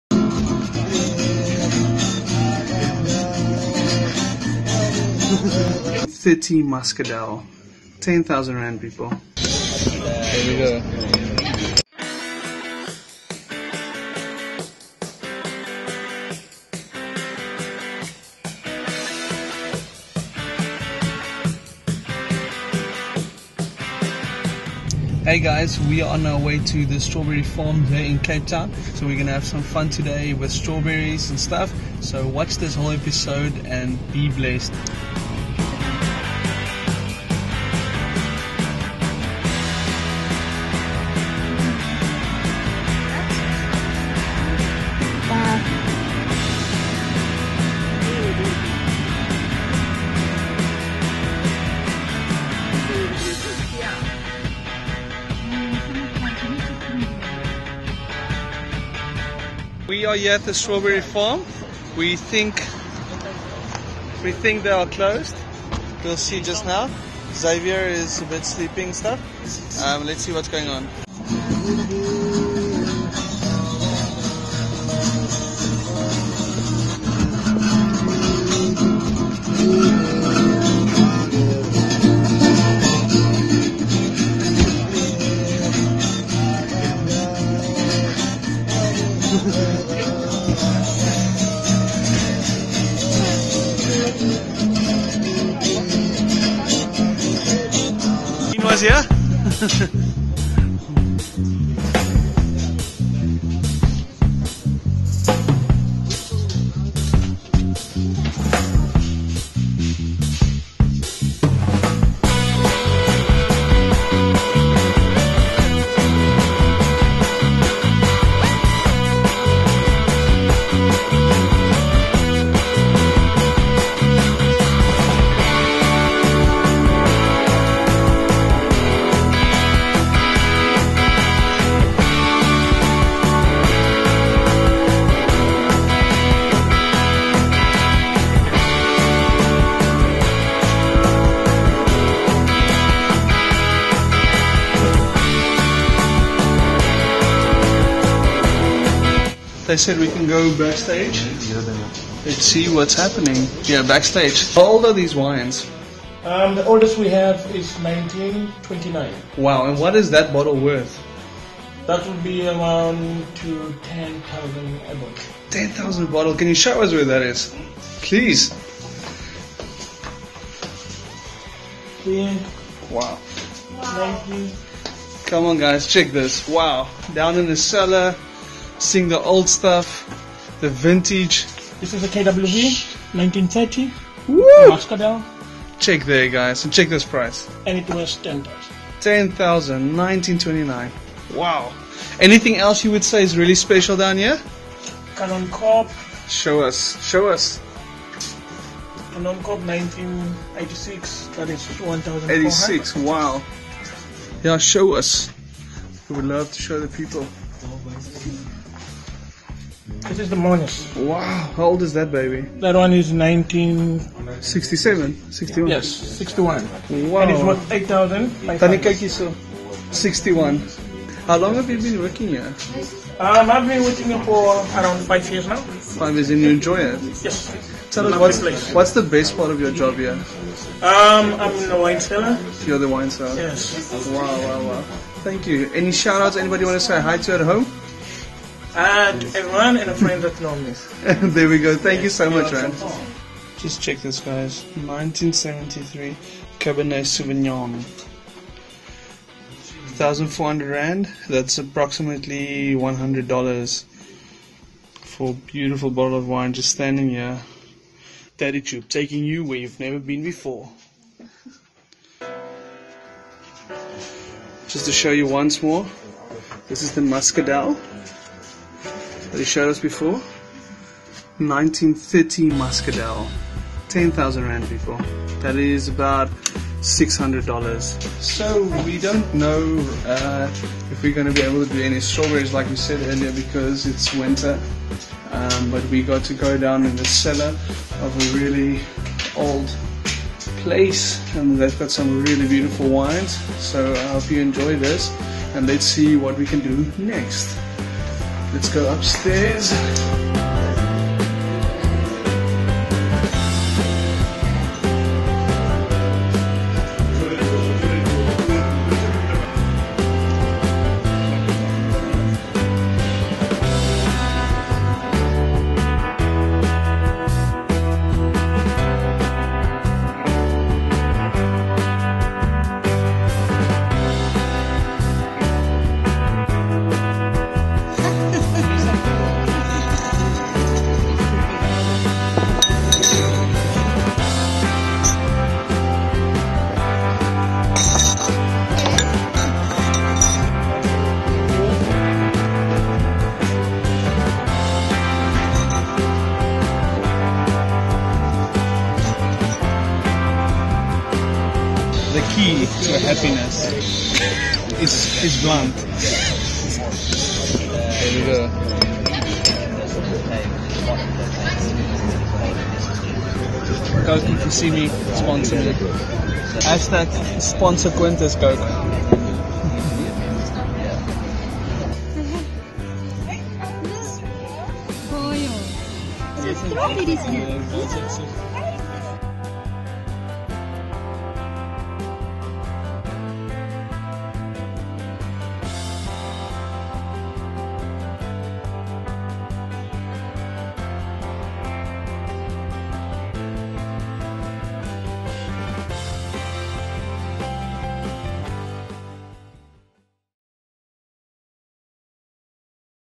Most Expensive Bottle of Wine
Most Expensive Bottle of Wine is an episode where we start of with going to a Strawberry Farm and ending up seeing the most expensive and oldest wines we will probably ever see.

It was such an amazing day to experience these moments with friends and family. Follow along as we explore these places!!!